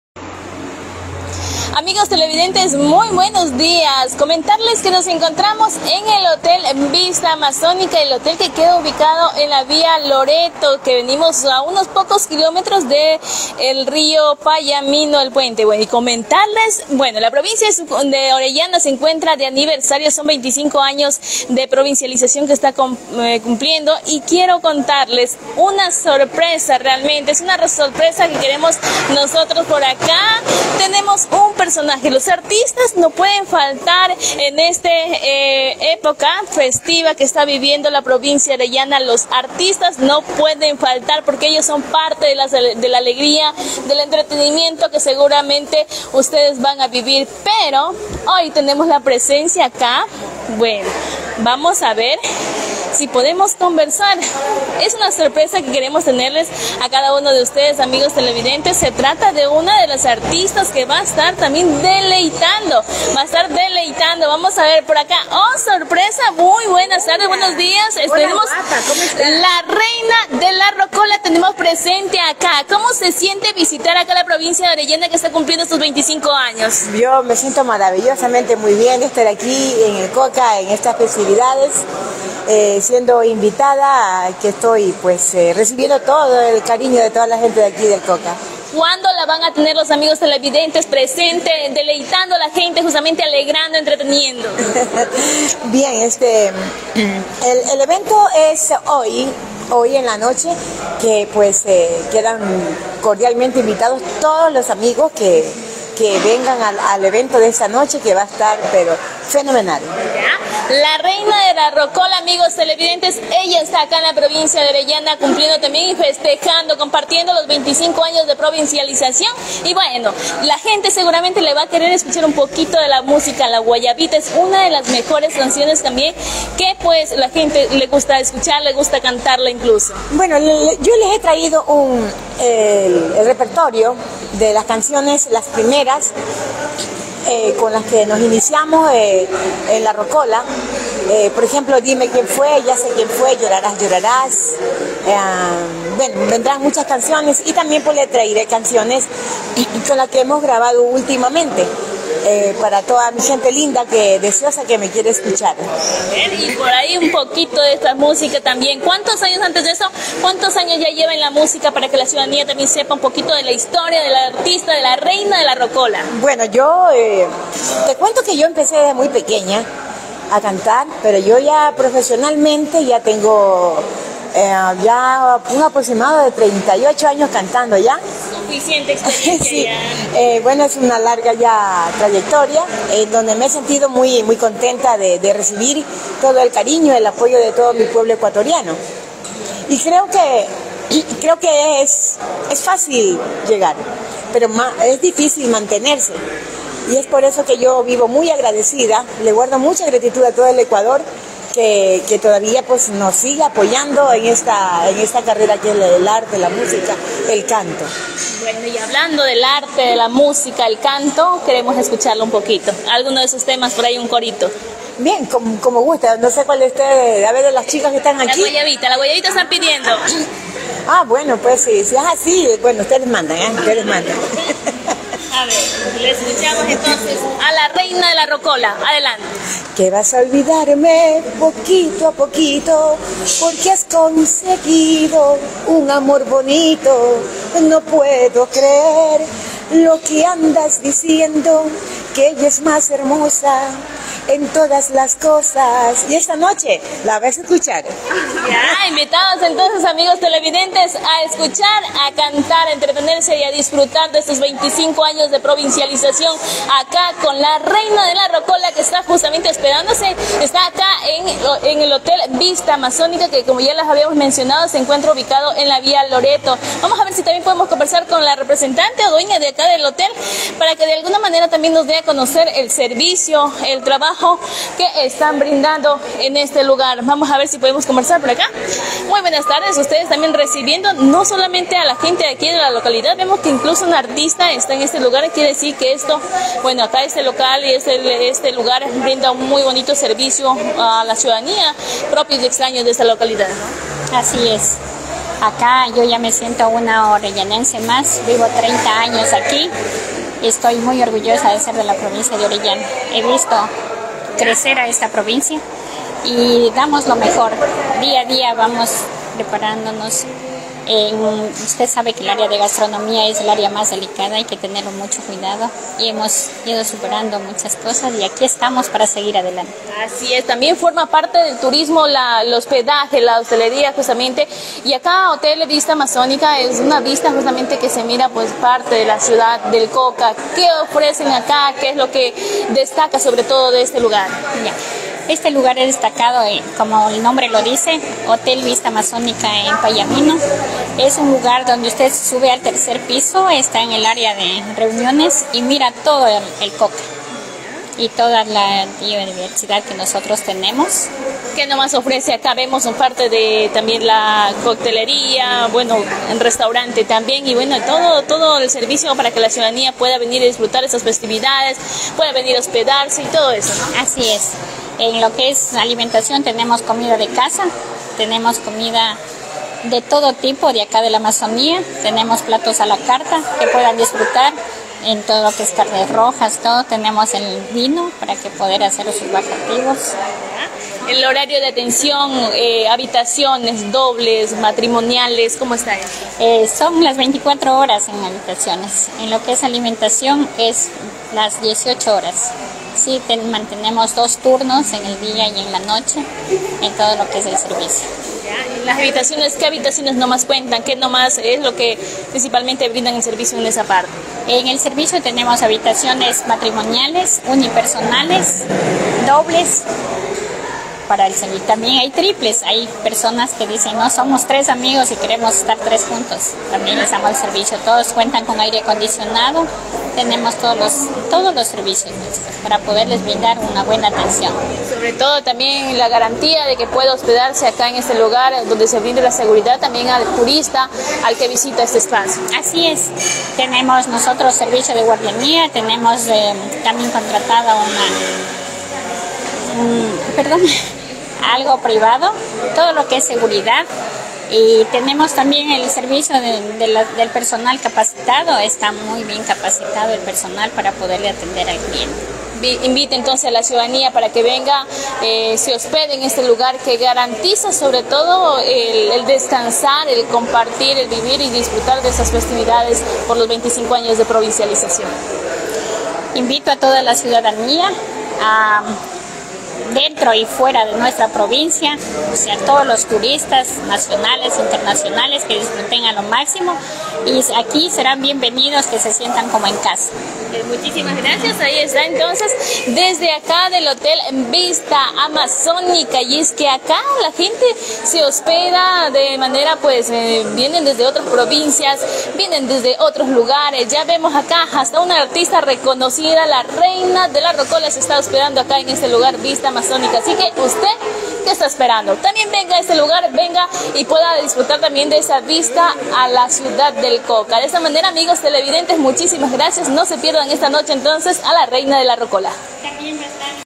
Jenny Rosero - Reina de la Rocola
EnVivo | Jenny Rosero la reina de la Rocola en vivo por los 25 años de provincialización de Orellana